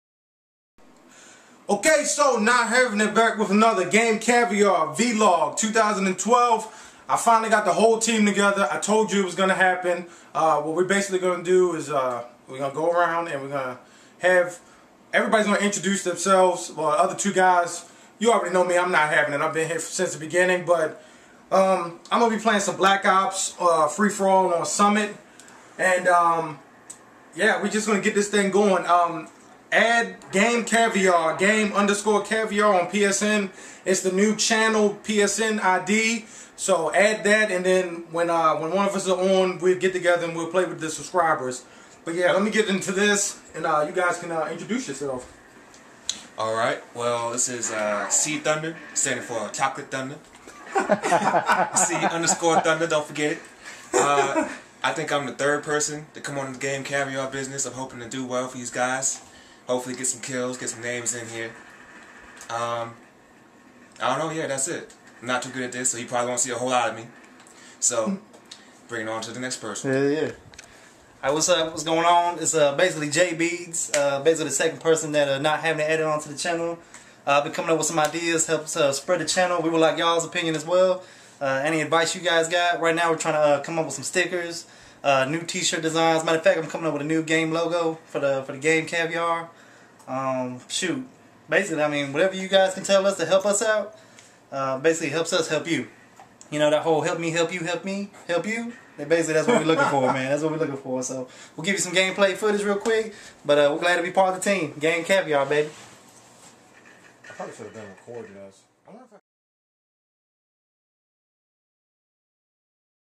Game Caviar Team Introductions
Here we have the whole team together and everyone introduces themselves. We also discuss some of the ways we are getting our name out and putting more eyes on our brand.